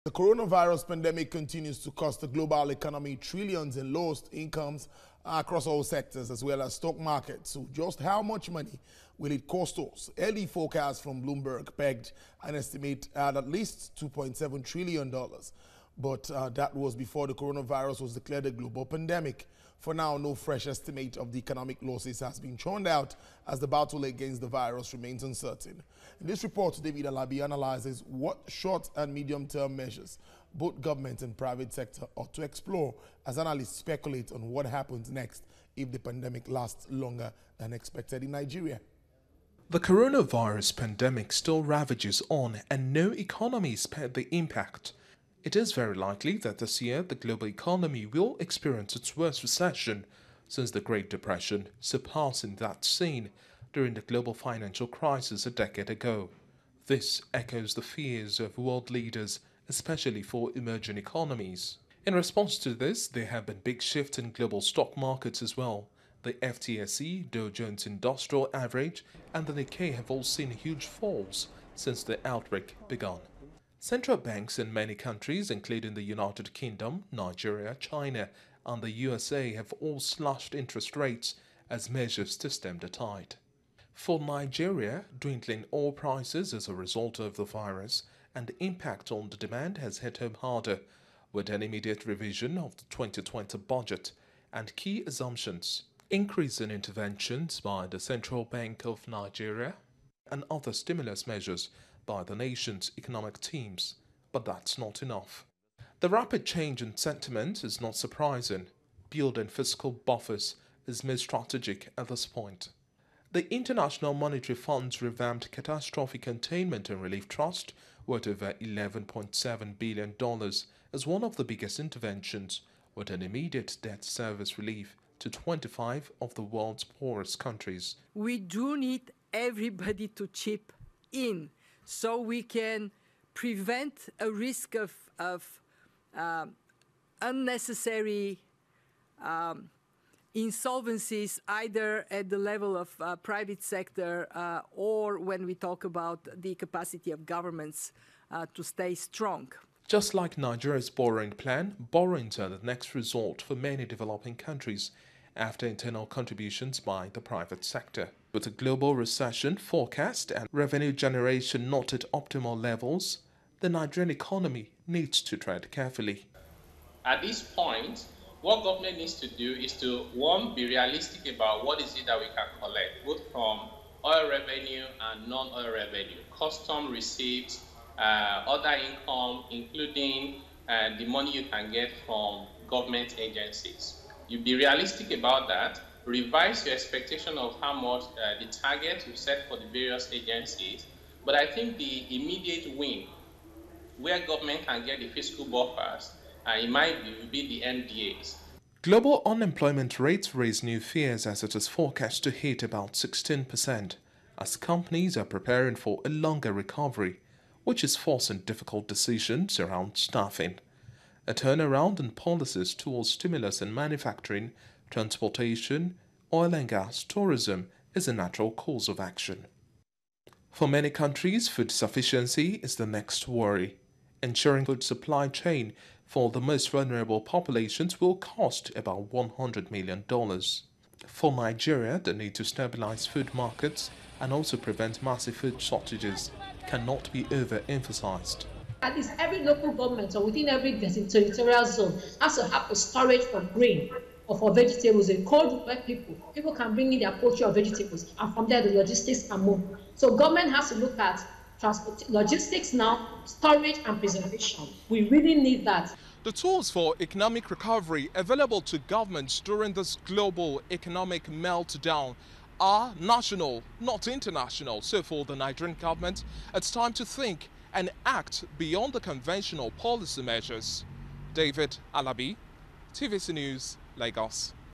COVID-19: The possible short, medium and long term measures to manage the economy
In this report, David Alabi looks at the possible short, medium and long term measures to manage the Nigerian Economy as the country battles the spread of COornavirus.